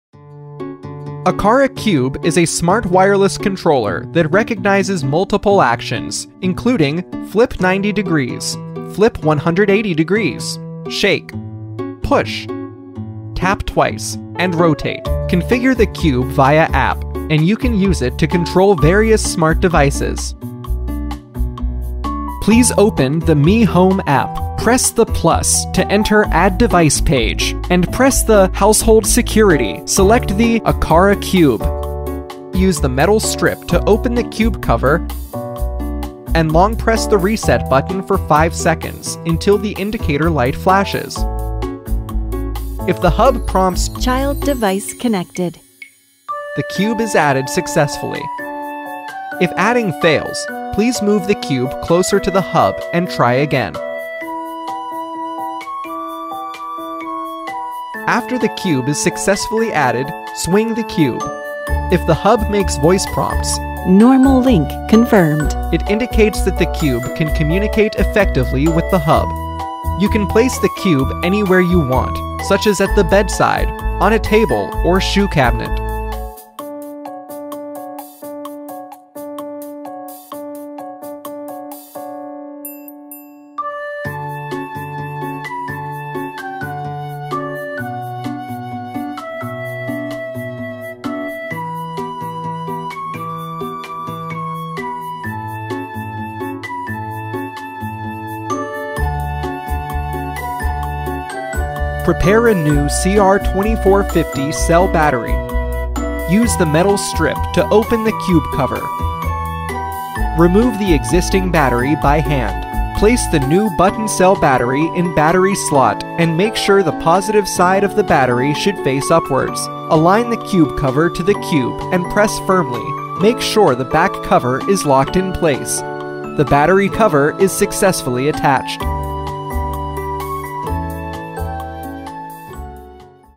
How to install the Aqara Cube with Mi Home app
Quick introduction and tutorial about the Aqara Cube with Mi Home app.